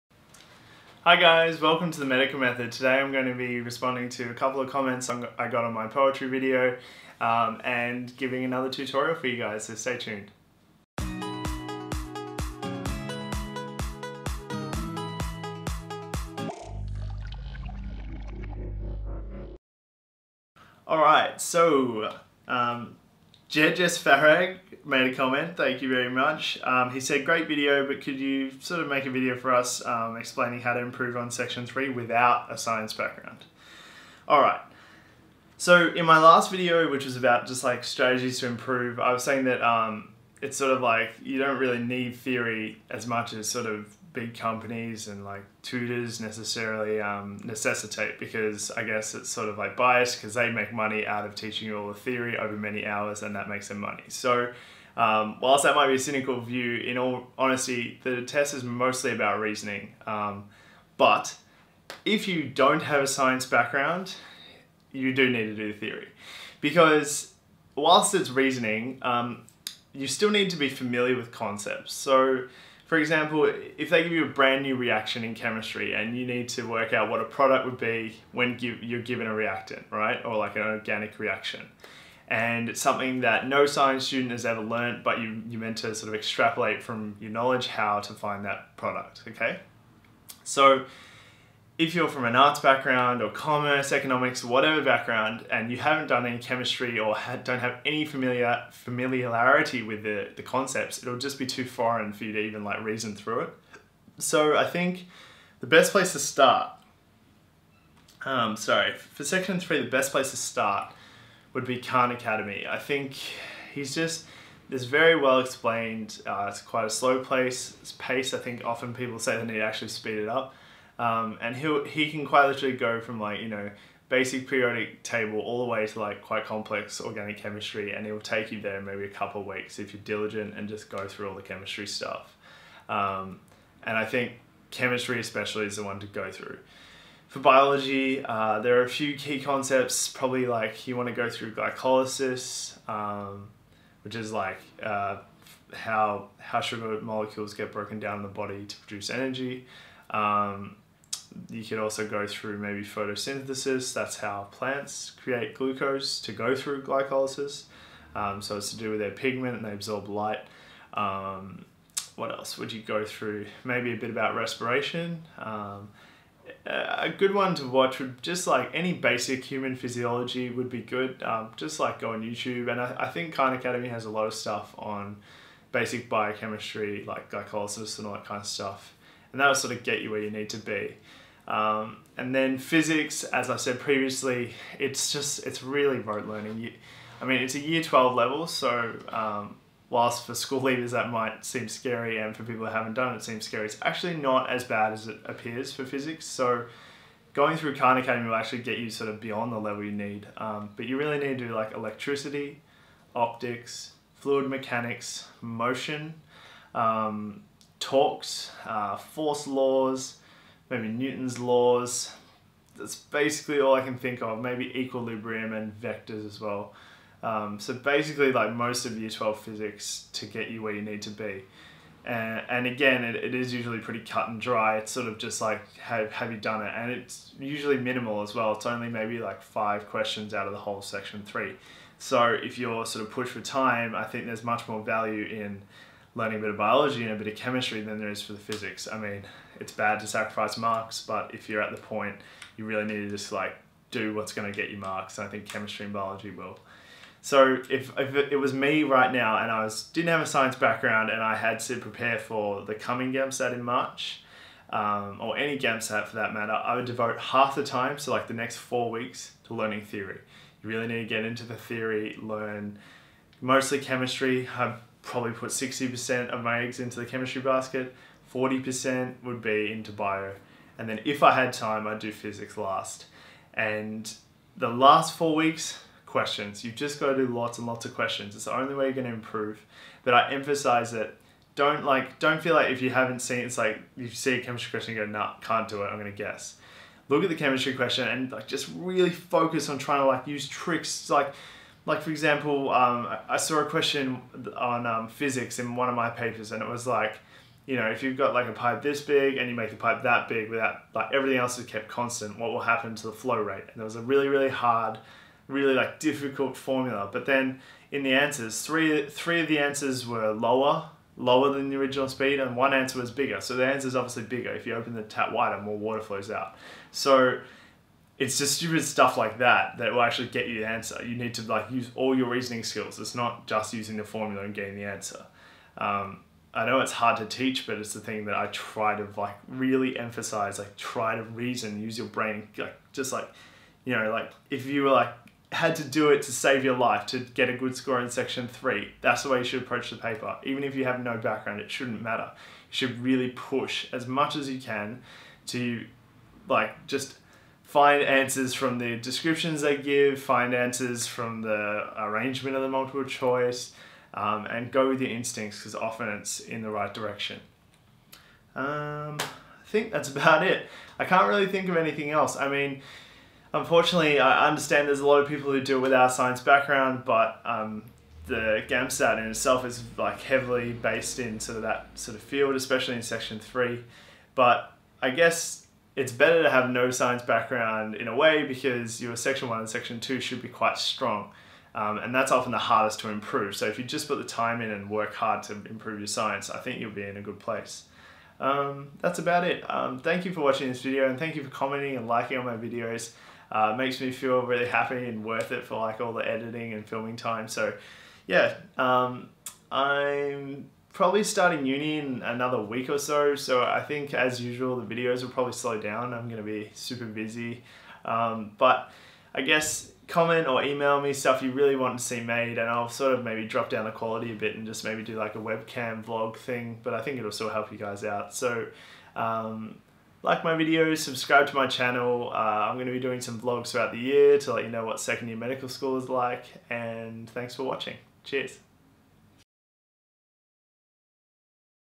Studying with a Non-Science Background - GAMSAT Advice
This is my GAMSAT tutorial for those with a Non-Science background.
Comment, Like and let me know what you want to see next.

Don't hesitate to get in contact with me there.